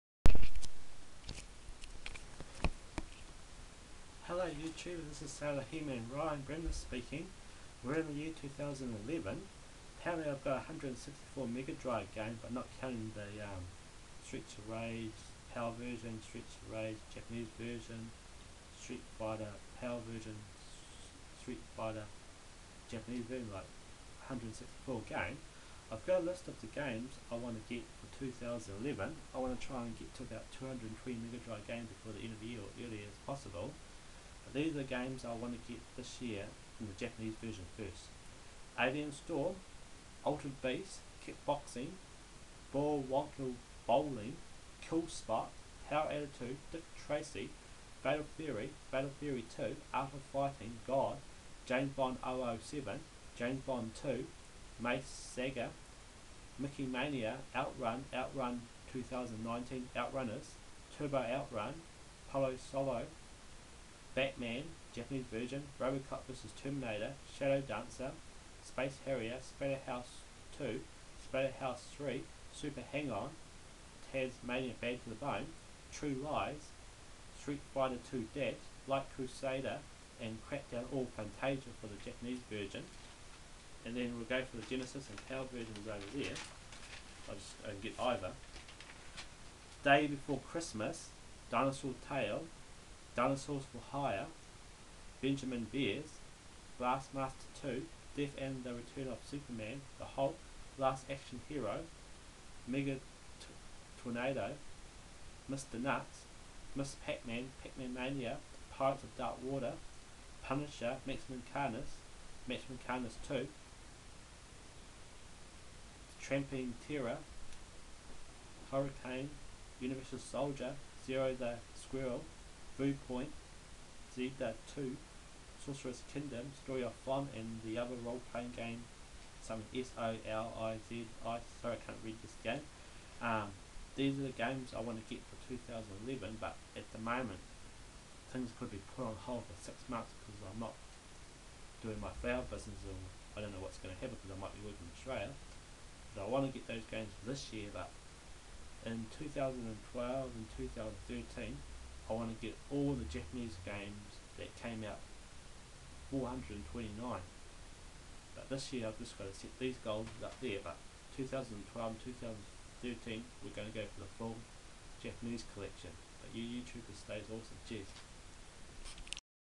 Mega Drive Update of Games me buying in 2011
Hi Youtuber I have 164 different Mega drive games.Not counting the double verison games.This year I want get about 56 more Mega Drive games in 2011 to make our new Target end 2011 about 220 Mega Drive Games.Plus I talk about new Target for 2012 to 2013.Cheers Ryan